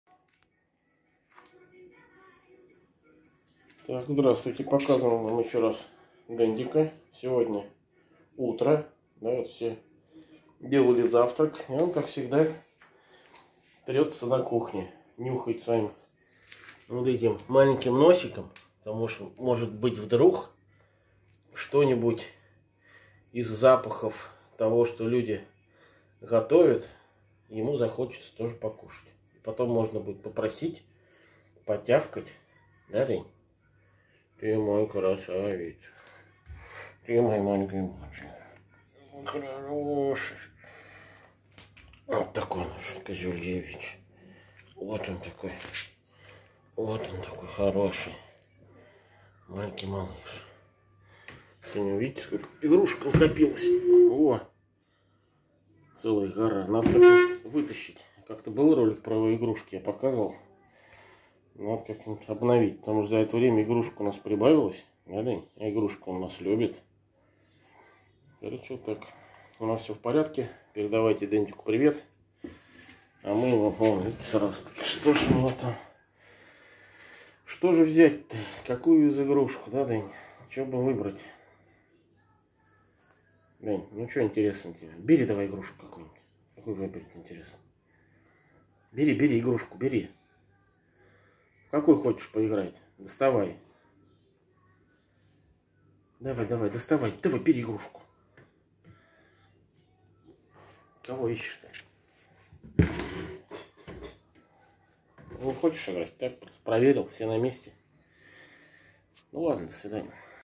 Пекинес Дендик с утра ждёт на кухне вкусняшек
Утром пекинес Дендик пребывает на кухне и оценивает есть ли что вкусное. Чтобы потом поспрашивать угощения.